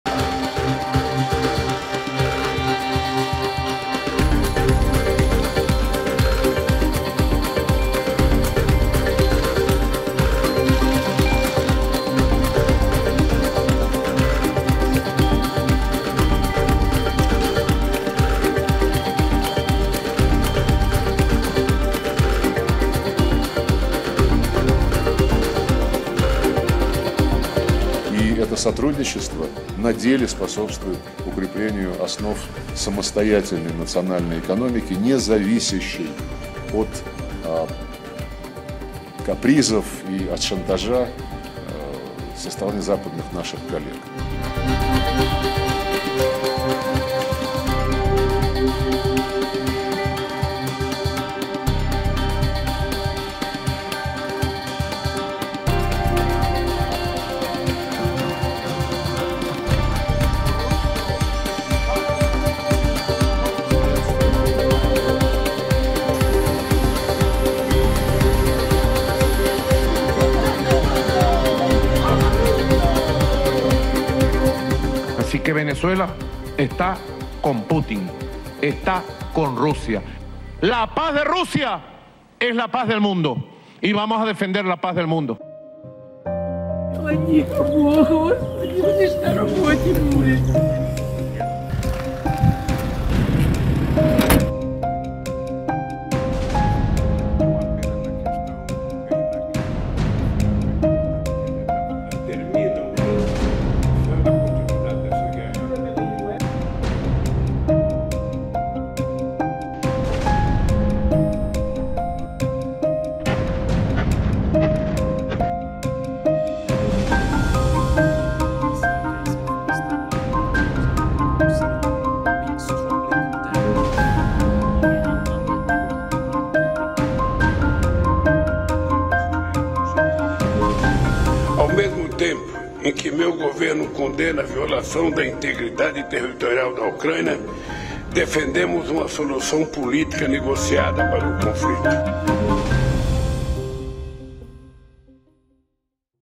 Venezuela y Rusia unidas contra el "chantaje y la presión" de Occidente
Según el ministro de Exteriores ruso, Serguéi Lavrov, ambos países están cooperando para enfrentar las sanciones de Europa y EE. UU. El canciller visitó recientemente Brasil. Allí fue recibido por el presidente Lula. Tras ser criticado por culpar a EE.UU. y Europa de prolongar la guerra en Ucrania, el mandatario brasileño finalmente condenó la invasión ordenada por Moscú.